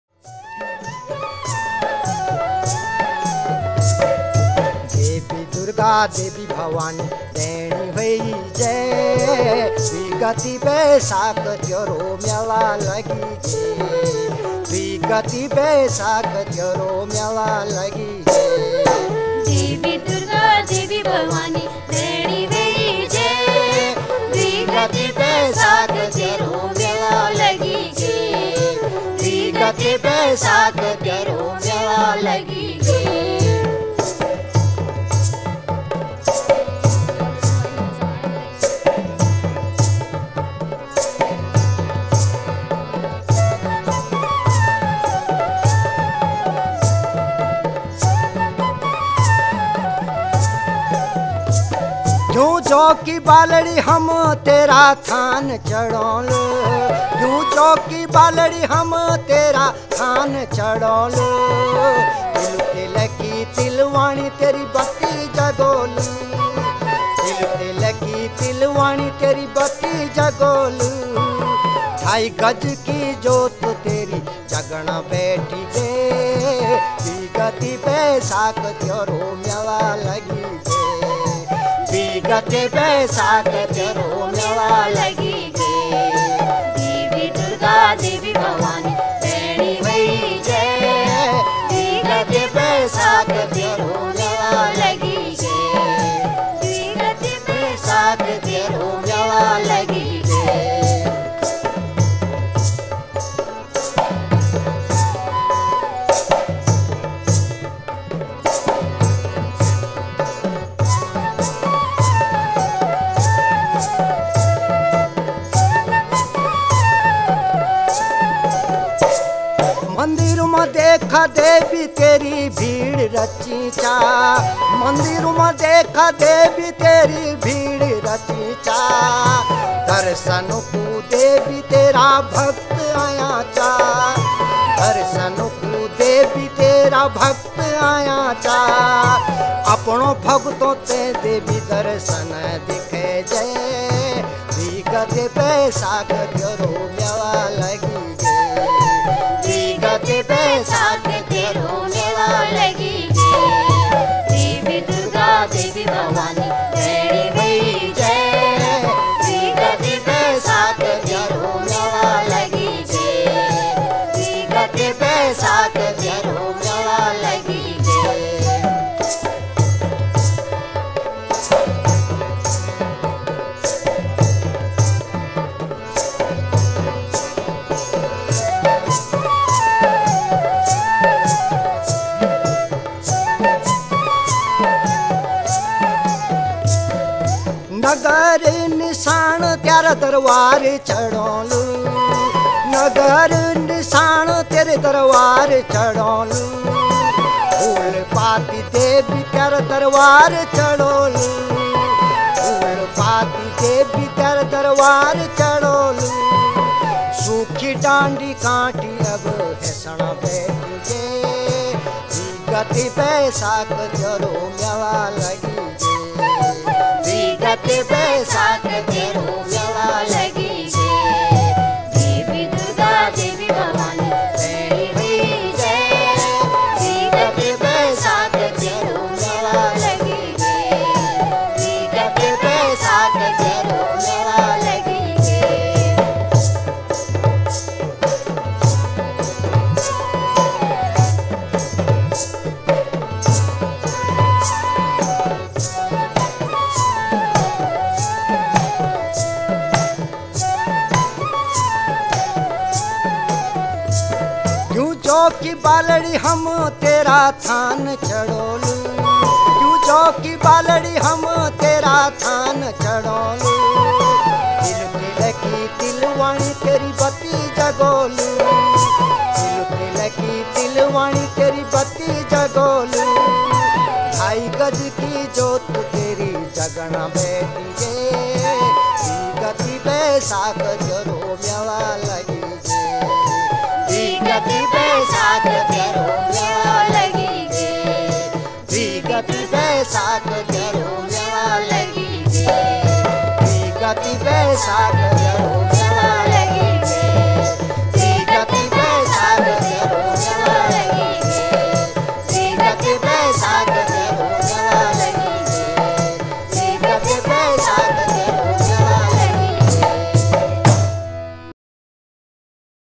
गीत संगीत- घनश्याम गैरोला ||GHANSHYAM GAIROLA, GAIRSAIN
गीत संगीत-  02
1. घनश्याम गैरोला- मुख्य गायक
2. दर्शन सिंह -सह गायन
3. राजलक्ष्मी देवी- कोरस
4. रेणु डोभाल-कोरस
5.  जितेन्द्र काला-हारमोनियम
6. कैलाश चन्द्र ध्यानी- बाँसुरी
7. जयेन्द्र सिंह-ढ़ोलक
8. आनन्द लाल शाह-हुड़का
9. वासुदेव शाह-साइड रिदम

प्रस्तुतकर्ता- विकास कोटनाला